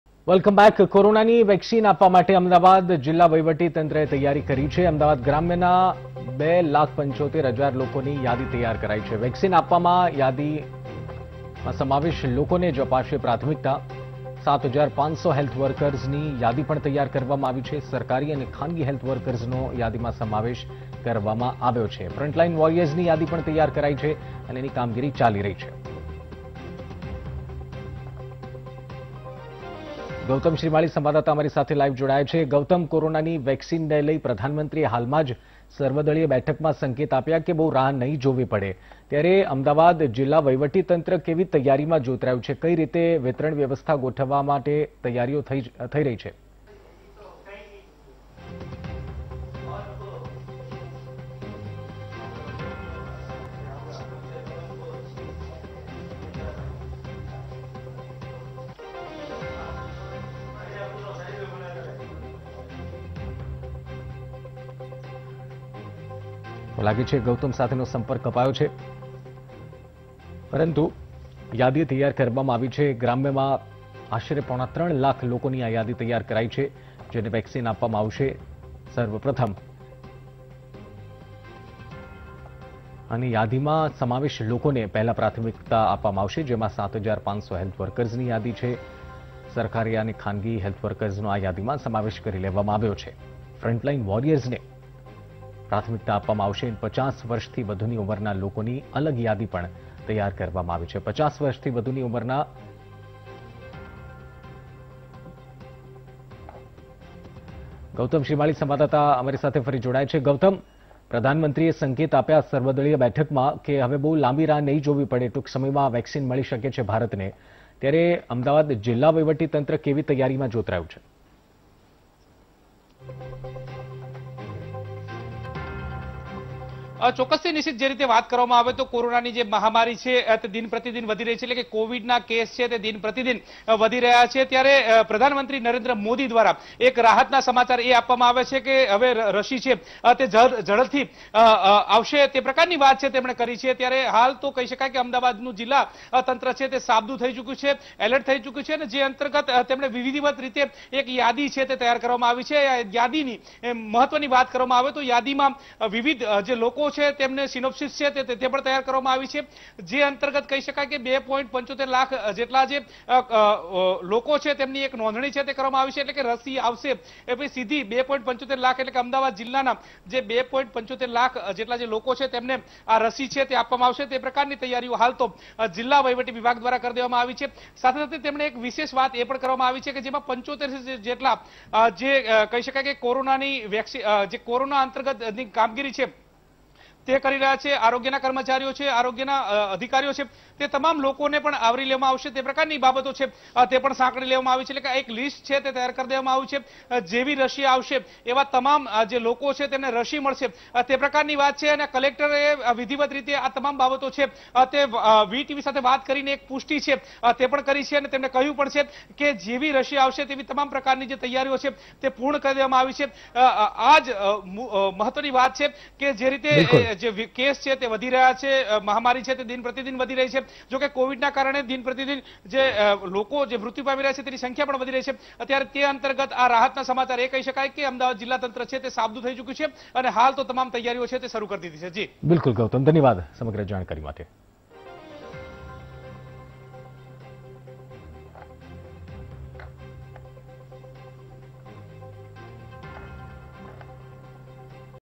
કોરોનાની વેક્સીન આપવા માટે તંત્રની તૈયારીઓ, સરકારી અને ખાનગી હેલ્થકેર વર્કર્સનો યાદીમાં સમાવેશ
કોરોનાની વેક્સીન આપવા માટે Ahmedabad તંત્રની તૈયારીઓ, સરકારી અને ખાનગી હેલ્થકેર વર્કર્સનો યાદીમાં સમાવેશ